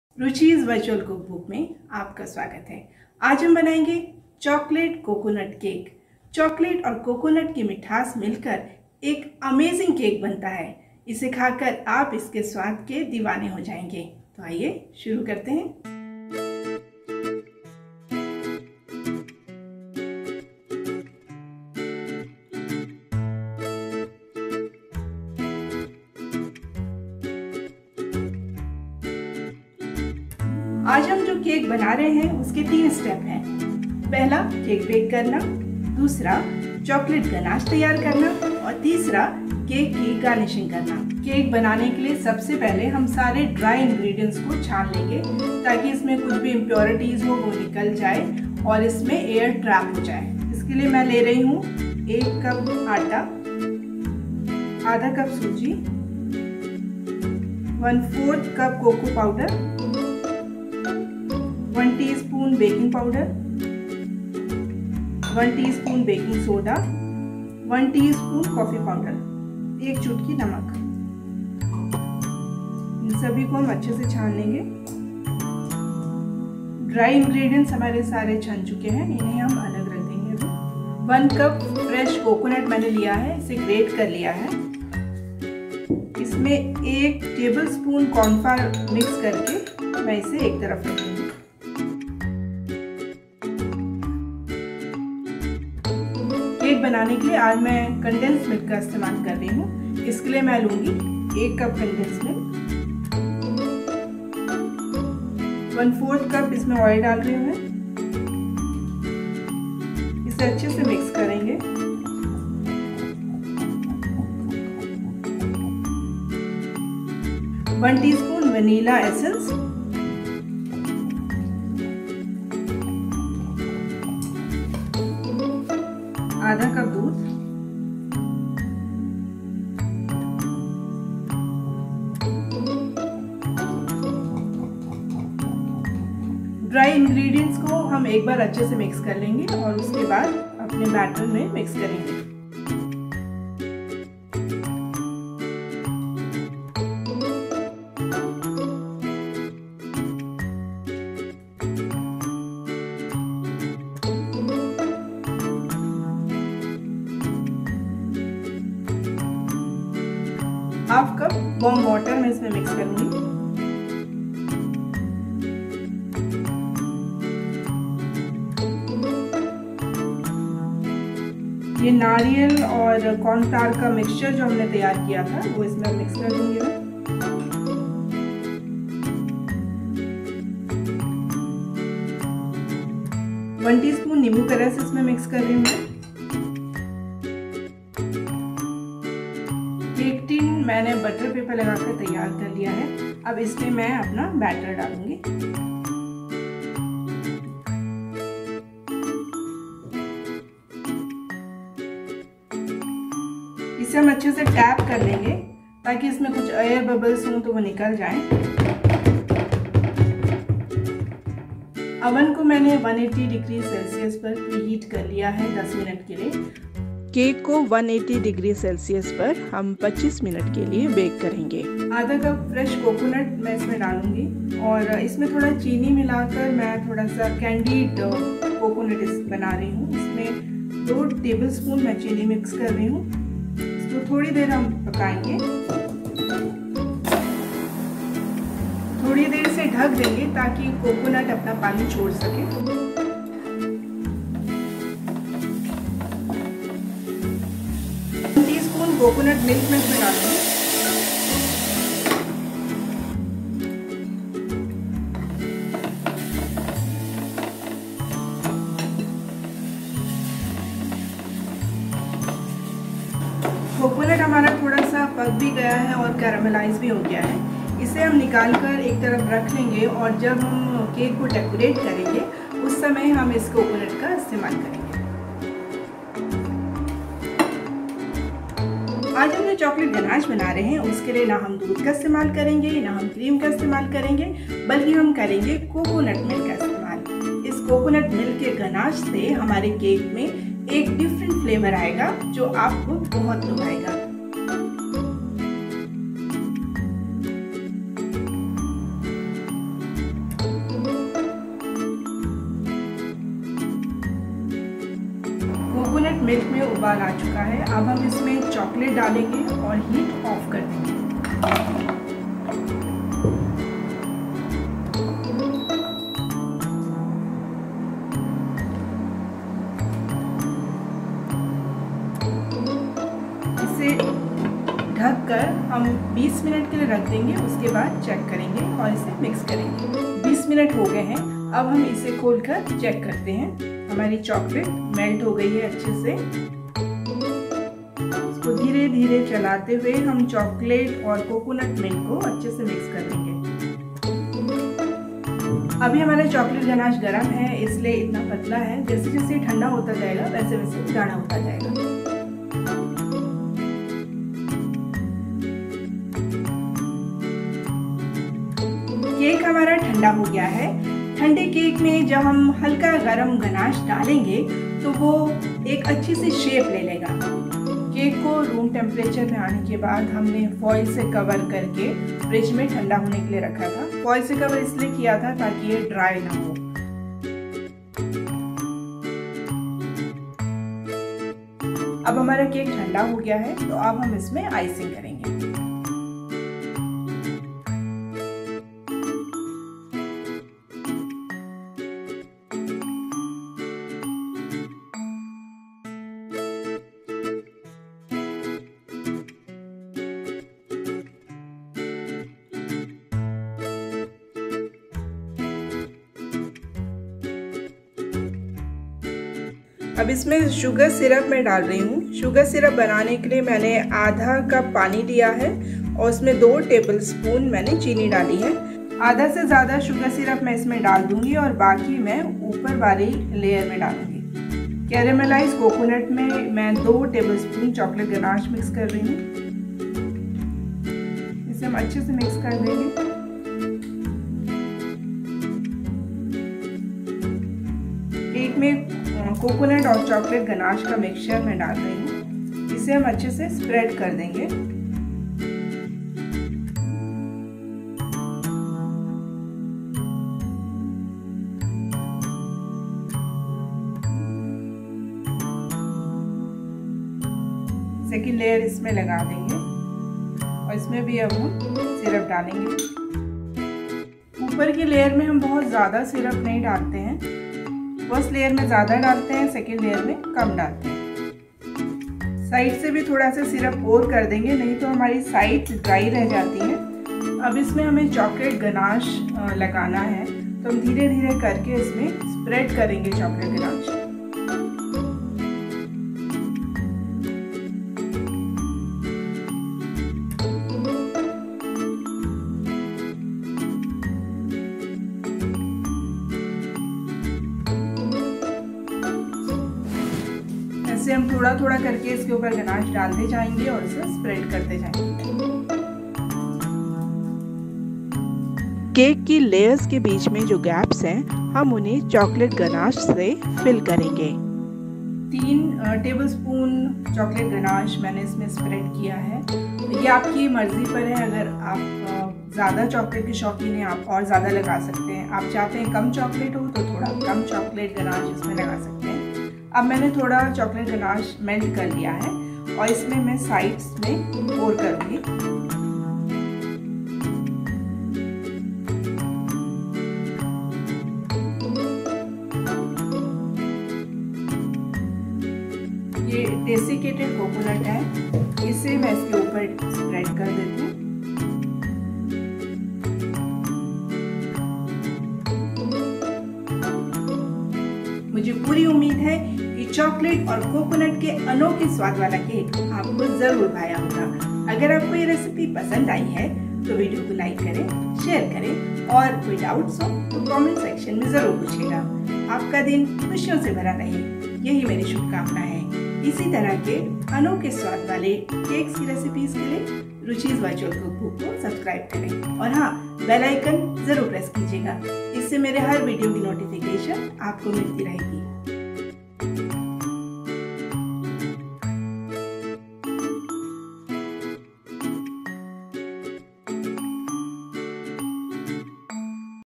Chocolate Coconut Cake | Eggless Cake Recipe
Chocolate Coconut is an amazing cake with the goodness and taste of Coconut and Chocolate. This Cake is a Fusion Cake that combines our Traditional Culture and ingredients with modern ones. It's a way to introduce and develop  the liking of coconut in our Chocolate Loveing People. This is an excellent way to make them like and make them aware of the goodness of Coconut. You can indulge in this cake. The recipe I'm sharing today is very easy to make. The best part of the recipe is that it's Eggless, therefore even vegetarian people can also enjoy it. I'm sure you'll like the recipe. Do try the recipe and share your experiences with me!